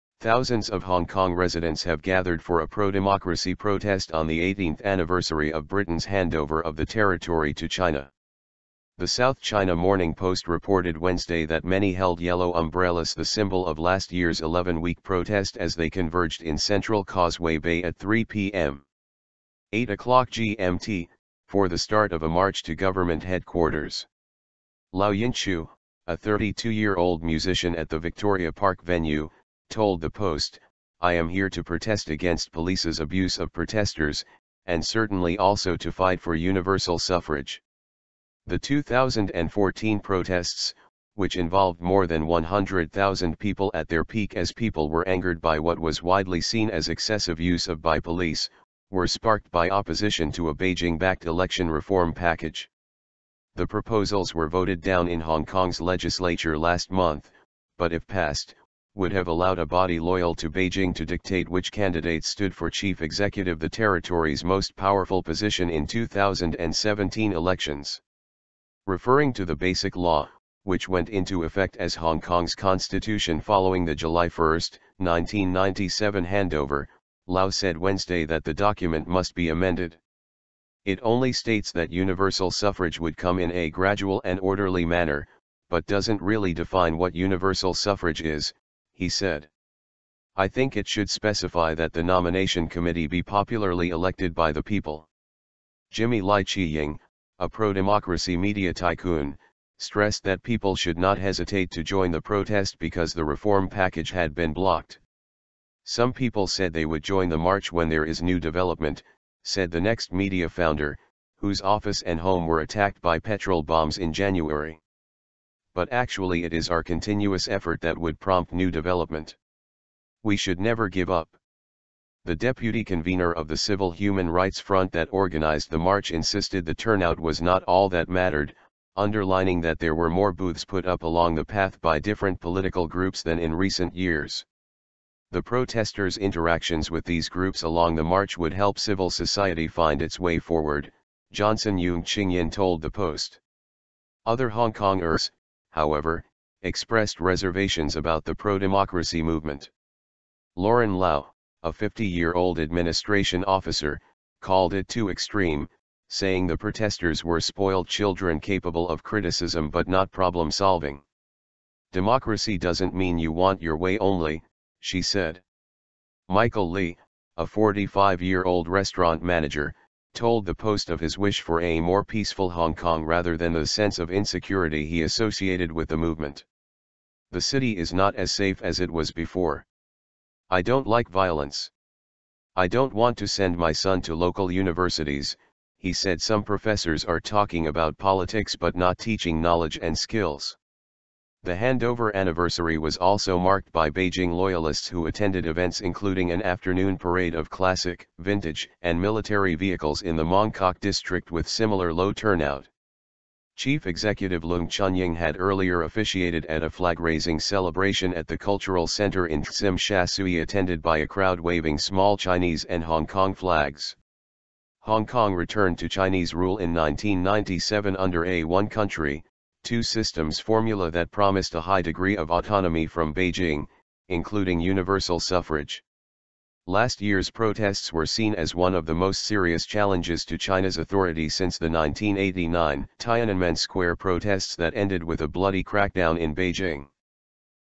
Hongkongers protest on anniversary of China handover
Thousands of Hong Kong residents have gathered for a pro-democracy protest on the 18th anniversary of Britain’s handover of the territory to China.

The South China Morning Post reported Wednesday that many held yellow umbrellas -- the symbol of last year's 11-week protest -- as they converged in central Causeway Bay at 3 p.m. (0800GMT) for the start of a march to government headquarters.

Lau Yin-chiu, a 32-year-old musician at the Victoria Park venue, told the Post, "I am here to protest against police’s abuse of protesters, and certainly also to fight for universal suffrage."

The 2014 protests, which involved more than 100,000 people at their peak as people were angered by what was widely seen as excessive use of by police, were sparked by opposition to a Beijing-backed election reform package.

The proposals were voted down in Hong Kong’s legislature last month, but if passed, would have allowed a body loyal to Beijing to dictate which candidates stood for chief executive -- the territory's most powerful position -- in 2017 elections.

Referring to the Basic Law, which went into effect as Hong Kong’s constitution following the July 1, 1997 handover, Lau said Wednesday that the document must be amended.

"It only states that universal suffrage would come in a 'gradual and orderly manner', but doesn’t really define what universal suffrage is," he said. "I think it should specify that the nomination committee be popularly elected by the people."

Jimmy Lai Chee-ying, a pro-democracy media tycoon, stressed that people should not hesitate to join the protest because the reform package had been blocked.

"Some people said they would join the march when there is new development," said the Next Media founder, whose office and home were attacked by petrol bombs in January.

"But actually it is our continuous effort that would prompt new development. We should never give up."

The deputy convener of the Civil Human Rights Front that organized the march insisted the turnout was not all that mattered, underlining that there were more booths put up along the path by different political groups than in recent years.

"The protesters’ interactions with these groups along the march would help civil society find its way forward," Johnson Yeung Ching-yin told the Post.

Other Hongkongers, however, expressed reservations about the pro-democracy movement.

Loren Lau, a 50-year-old administration officer, called it "too extreme", saying the protesters were "spoiled children" capable of criticism but not problem-solving.

"Democracy doesn’t mean you want your way [only]," she said.

Michael Lee, a 45-year-old restaurant manager, told the Post of his wish for a "more peaceful Hong Kong" rather than the "sense of insecurity" he associated with the movement.

"The city is not as safe as it was before. I don’t like violence. I don’t want to send my son to local universities," he said "Some professors are talking about politics but not teaching knowledge and skills."

The handover anniversary was also marked by Beijing loyalists who attended events -- including an afternoon parade of classic, vintage and military vehicles in the Mong Kok district -- with similar low turnout.

Chief Executive Leung Chun-ying had earlier officiated at a flag-raising celebration at the cultural centre in Tsim Sha Tsui attended by a crowd waving small Chinese and Hong Kong flags.

Hong Kong returned to Chinese rule in 1997 under a "one country, two systems" formula that promised a high degree of autonomy from Beijing, including universal suffrage.

Last year’s protests were seen as one of the most serious challenges to China's authority since the 1989 Tiananmen Square protests that ended with a bloody crackdown in Beijing.